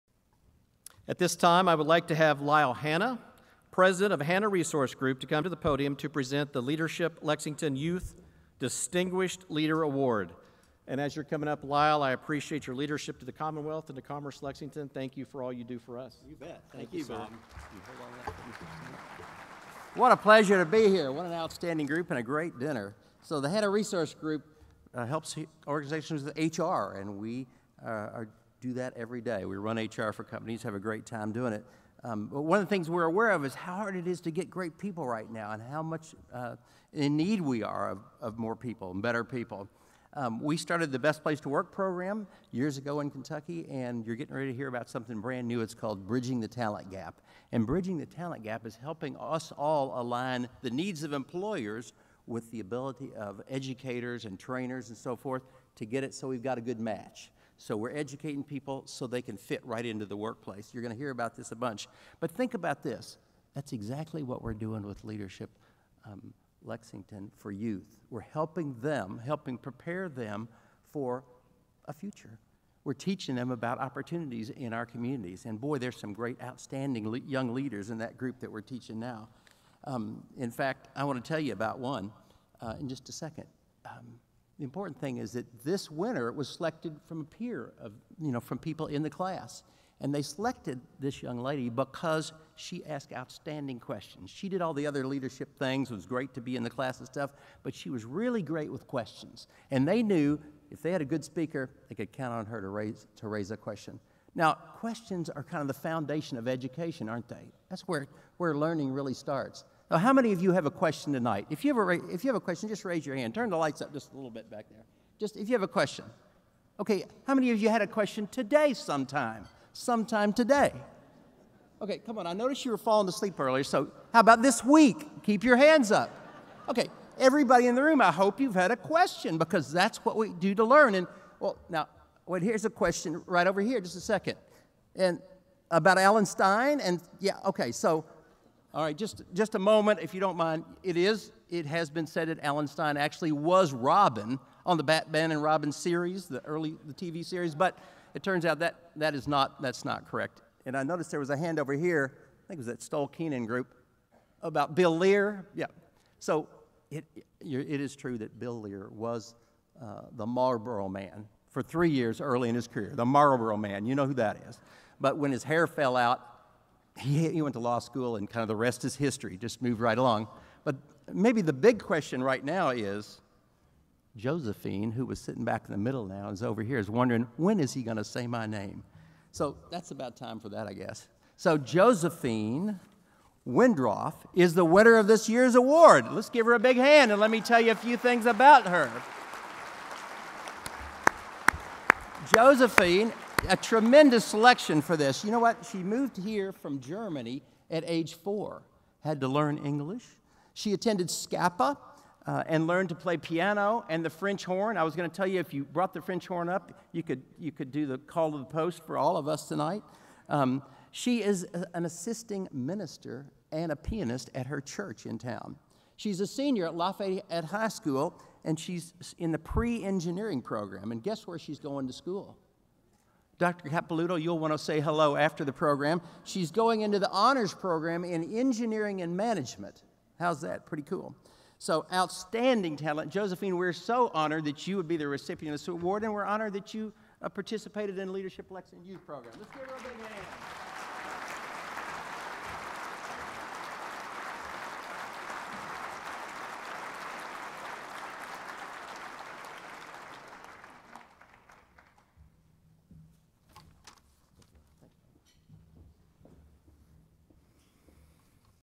Annual Dinner 2017: Josephine Wendroth
Josephine Wendroth from Lafayette High School received the Leadership Lexington Youth Program's Distinguished Leader Award at the Commerce Lexington Inc. Annual Dinner presented by Kentucky Employers' Mutual Insurance on January 26, 2017.